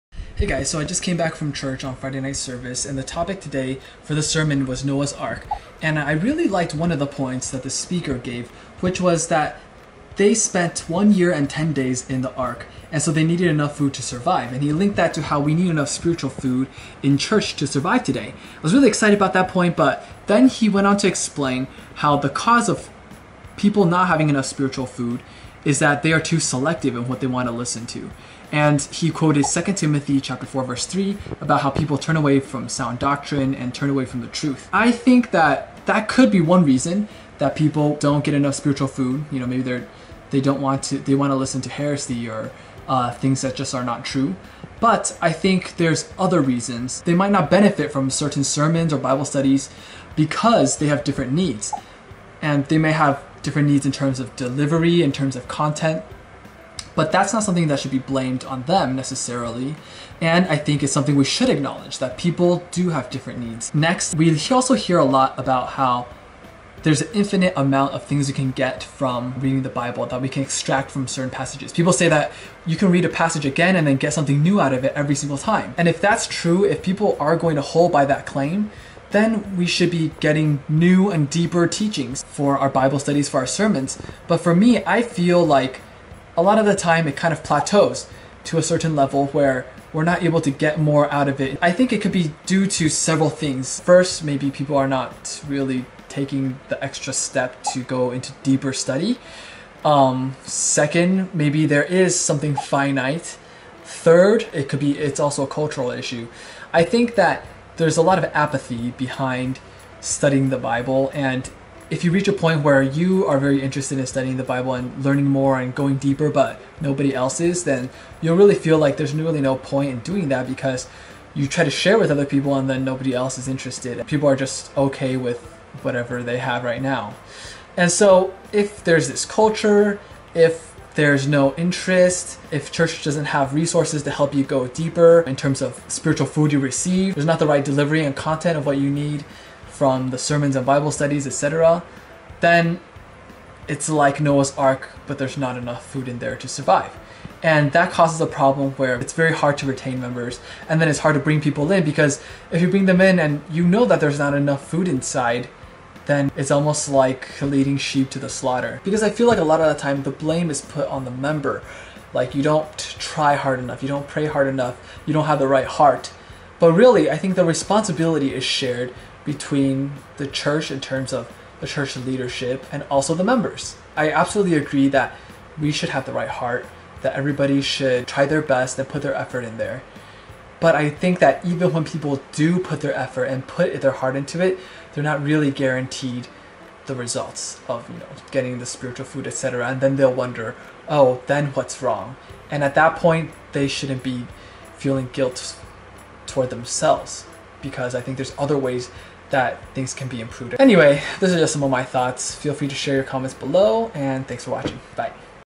Not Enough Spiritual Food In Church??
While the "lack of spiritual food" can be caused by member's being overly selective in what they want to hear, we should also consider the significance of member needs, general culture, and church leadership following through with providing strong resources for continual growth in spiritual knowledge.

There are certain personalities that are fine with church content just the way it is, but I think it's still very important to sincerely address all member needs and personalities.